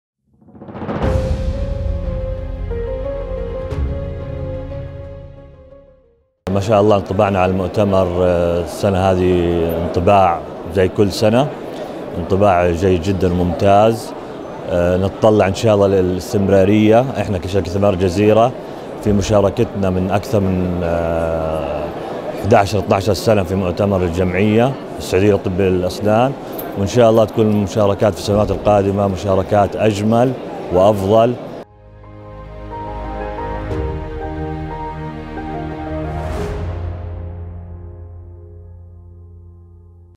مقابلة د. وسام شاهين، مدير قسم الأسنان في ثمار الجزيرة خلال المؤتمر السعودي العالمي لطب الأسنان 2020
Interview with Dr. Wisam Shahin – Dental Department Head at Thimar Al Jazirah Co. – during SIDC 2020